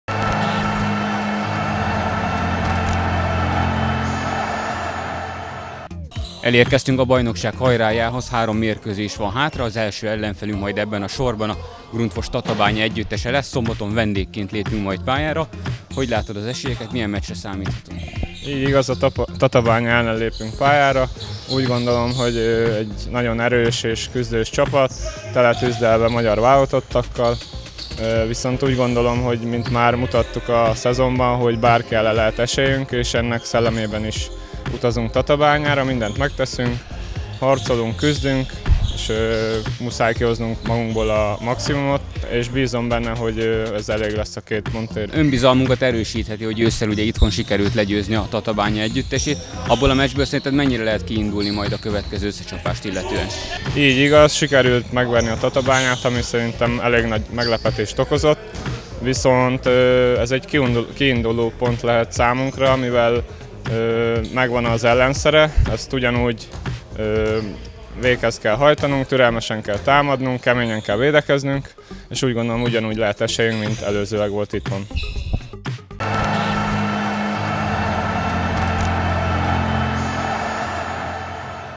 A Grundfos Tatabánya KC–SBS-Eger bajnoki mérkőzés előzetese
„Küzdeni fogunk a végsőkig, igyekszünk kihozni magunkból a maximumot, s bízunk benne hogy ez elég lehet egy újabb meglepetéshez” – jelenlette ki csapatunk irányító játékosa, Schmid Péter, a szombaton 18 órakor kezdődő Grundfos Tatabánya KC–SBS-Eger bajnoki mérkőzés felvezetése kapcsán.